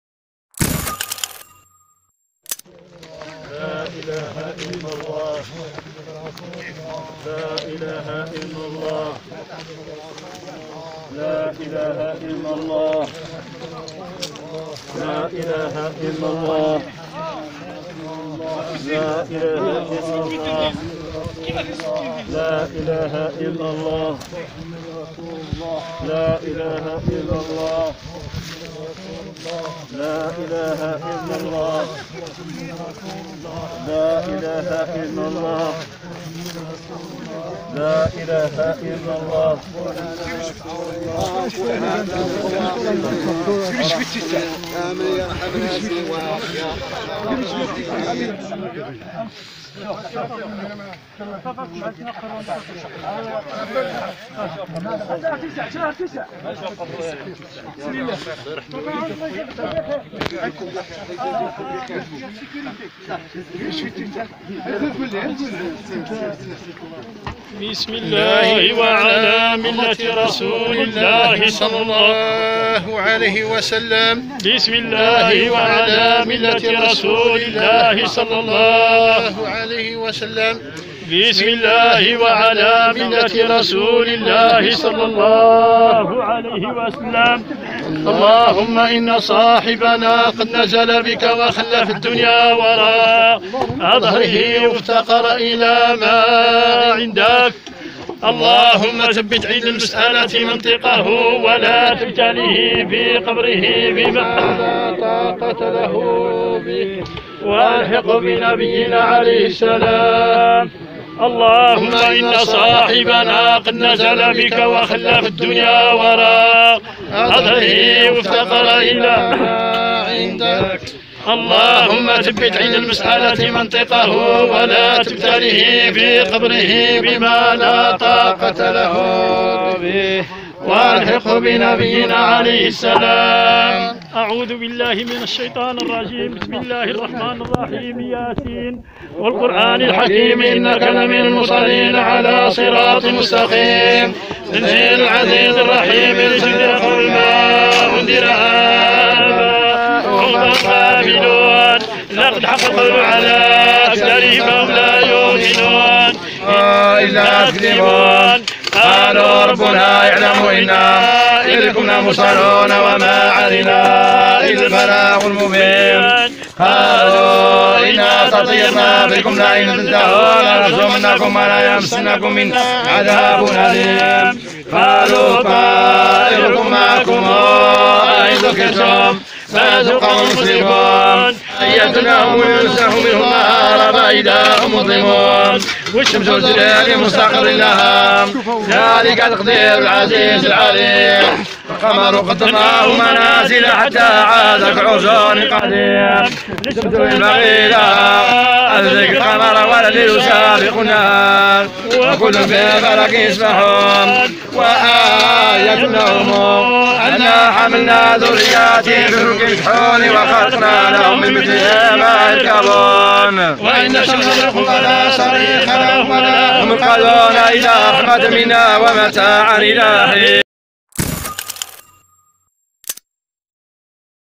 لحظة د-فن المرحوم المناضل الكبير المحجوبي احرضان
Alwankoum
فيديوهات مغربية
صوت وصورة